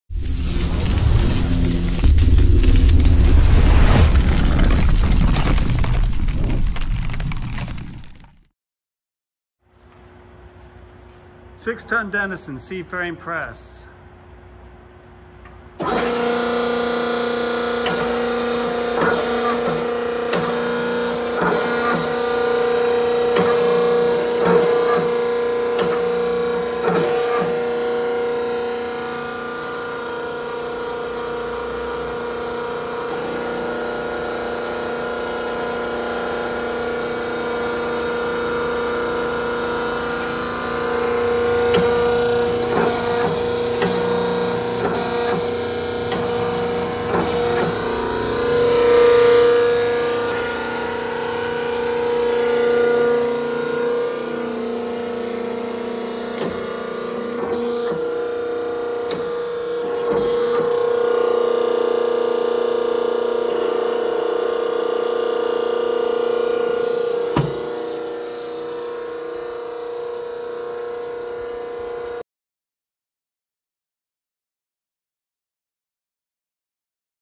6 TON DENISON C FRAME HYDRAULIC PRESS: STOCK #78020
SERIAL 23588-A201S3200789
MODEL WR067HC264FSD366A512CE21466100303
CAPACITY 6 TON
STROKE 12"
OPEN HEIGHT 16"
RAM SPEED - CLOSING 240 IPM
RAM SPEED - PRESSING 100 IPM
RAM SPEED - RETURN 265 IPM
RAM CYLINDER DIAMETER 2-1/4"
CYCLINDER BORE ... 3-1/4"
BED AREA 17" X 10-1/2"
D-SLOT IN BED 4-7/8" W X 8" L
THROAT 7-1/2
FLOOR TO BED 36"
PUMP CAPACITY 7 GPM
OPERATING PRESSURE 2020 PSI
DUAL PALM BUTTONS YES
MOTOR 7.5 HORSEPOWER